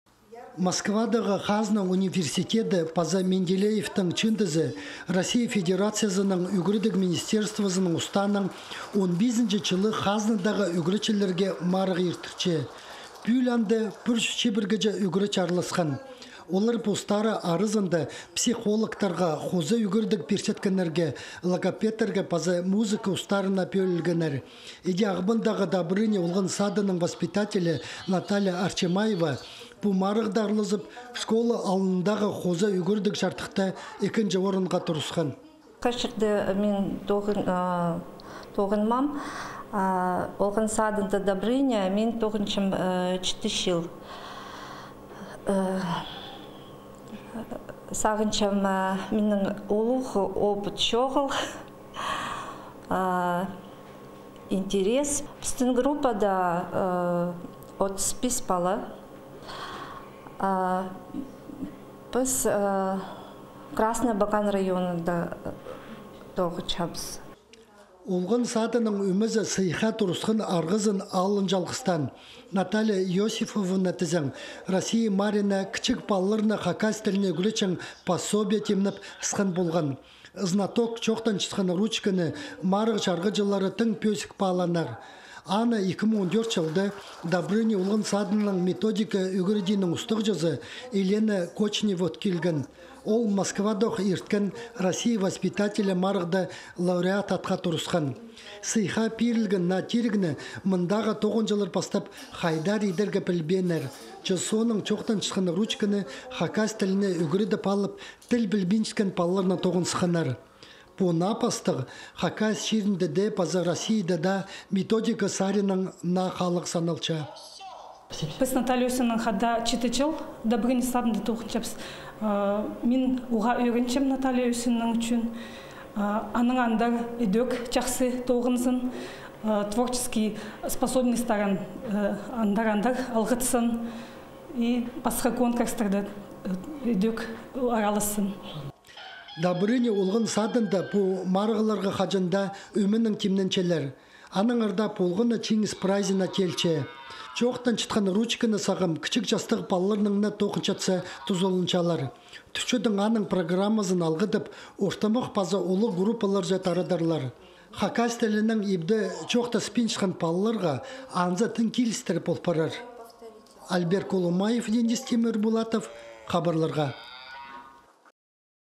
Воспитатель Абаканского детского сада «Добрыня» стала призёром Всероссийского конкурса
Агбандагы «Добрыня» олган садындагы  воспитатель Наталья  Арчимаева, угретчiлернiн, хазна син1ндегi мариинда аралазып, « Школа алнындагы хоза угредiг чардыхта» iкiнчi орынга турысхан.